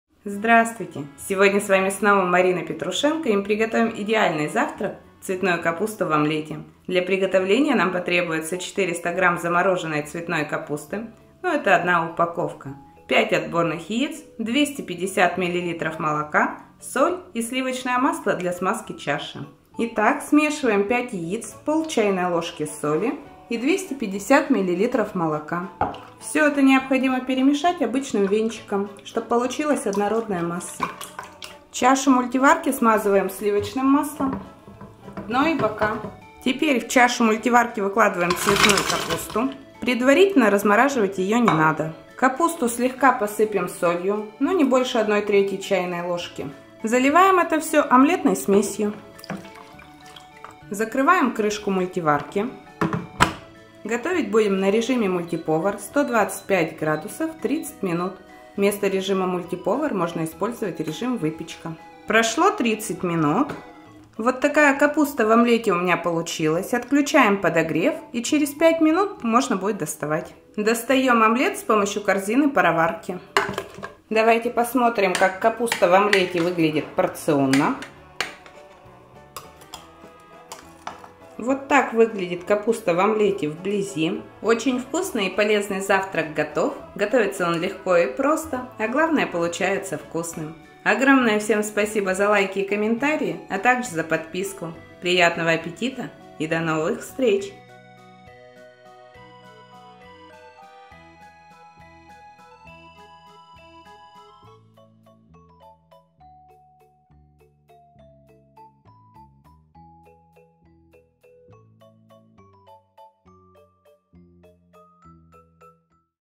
♨️ИДЕАЛЬНЫЙ ЗАВТРАК ОМЛЕТ С ЦВЕТНОЙ КАПУСТОЙ В МУЛЬТИВАРКЕ 🍳РЕЦЕПТ ОМЛЕТА РЕЦЕПТЫ ДЛЯ МУЛЬТИВАРКИ
Омлет с цветной капустой в мультиварке Redmond. Простой рецепт омлета пошагово. Как приготовить вкусный завтрак в домашних условиях. Рецепты для мультиварки. Мультиварка.

НИЖЕ ВКУСНАЯ ДОМАШНЯЯ ЕДА И ПРОСТЫЕ РЕЦЕПТЫ ДЛЯ МУЛЬТИВАРКИ 🔽🔽🔽

ОМЛЕТ С ЦВЕТНОЙ КАПУСТОЙ НА ЗАВТРАК
🍜 Состав:
400 гр. – цветной капусты
5 – отборных яиц
250 мл. – молока
Соль
Сливочное масло

ПРОСТОЙ РЕЦЕПТ ОМЛЕТА В МУЛЬТИВАРКЕ
⏰ Время приготовления:
Готовим 30 минут на режиме «выпечка» или «мультиповар 125С».